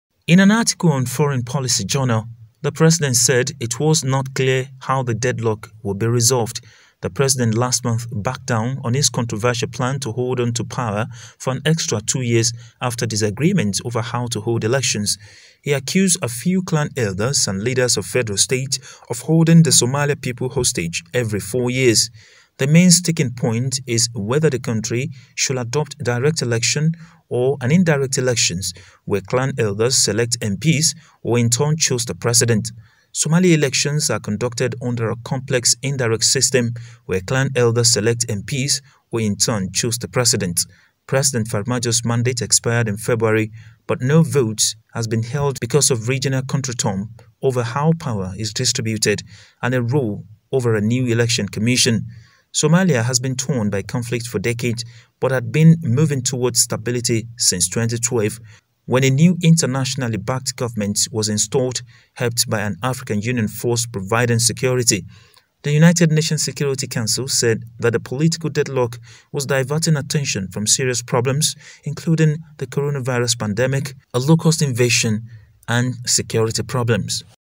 Somali president sidetracks blames over delay of election
Somalia’s President Mohamed Abdullahi Farmajo, has deflected accusations that he is clinging onto power by deliberately delaying overdue elections.